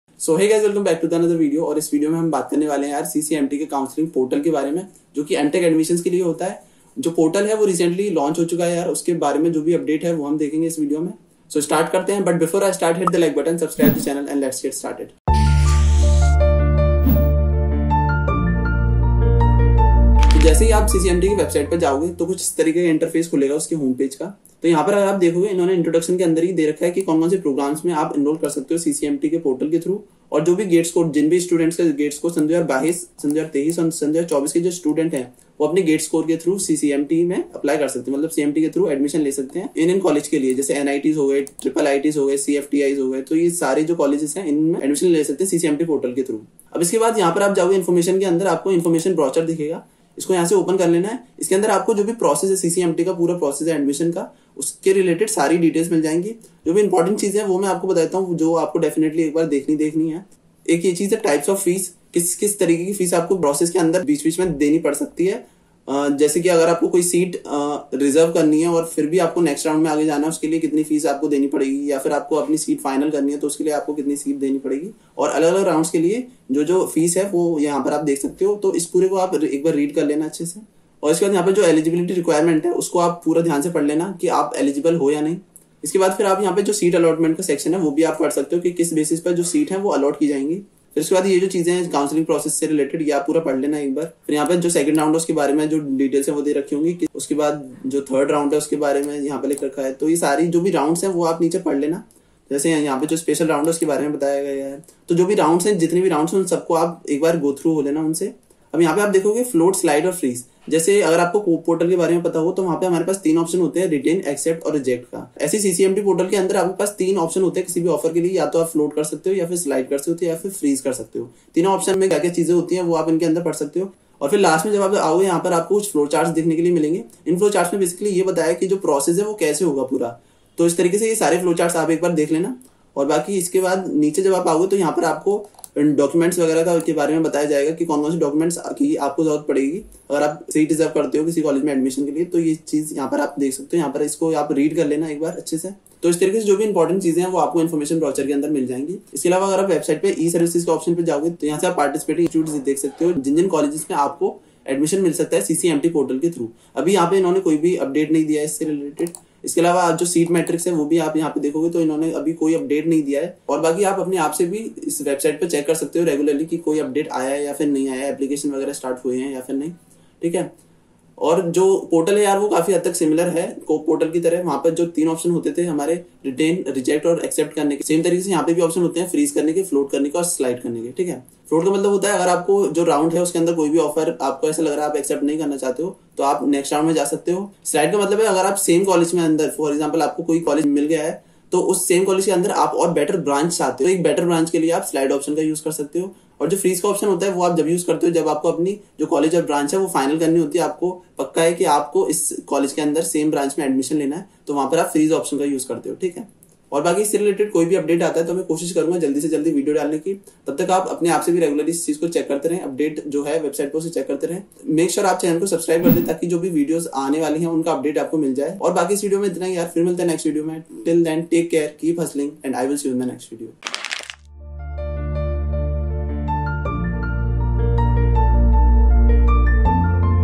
CCMT 2024 Portal | NIT MTech Admission | Important Update
In this video I have talked about the recently released CCMT portal for NITs and IIITs MTech Admissions 2024 and how you can choose your desired college and branch.